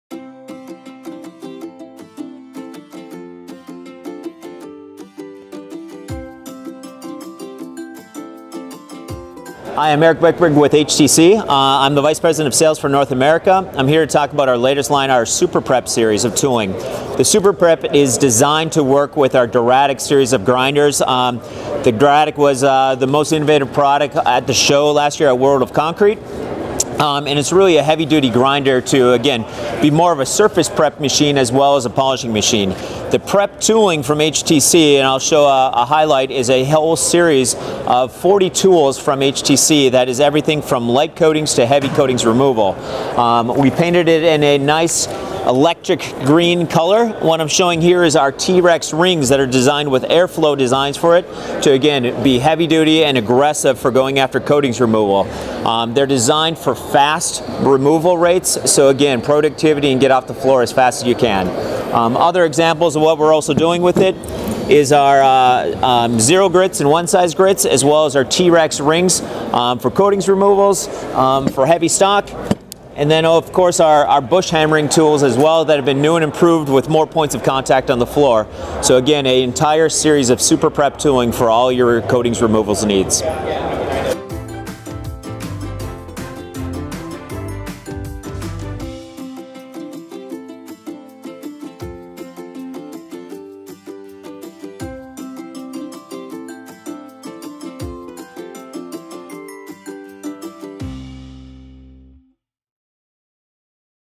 HTC Superprep™ tooling for removing old paint or levelling compound
HTC Superprep™ tooling is a series of 40 tools designed to work with DURATIQ series of grinders for coatings removal.